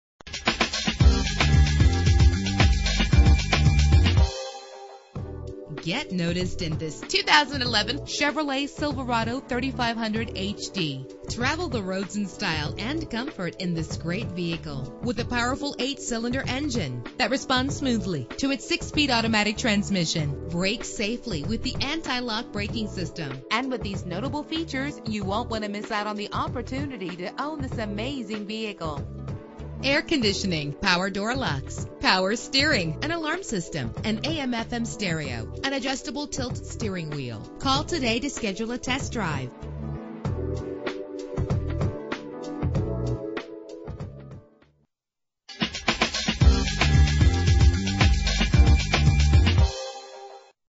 2011 Chevrolet Silverado 3500HD - Fayetteville AR
Year: 2011
Make: Chevrolet
Model: Silverado 3500HD
Trim: WT
Engine: 6.6 liter 8 cylinder 32 valve
Transmission: 6-Speed Automatic
Color: Summit White
Mileage: 0
Address:
1310 W Showroom Dr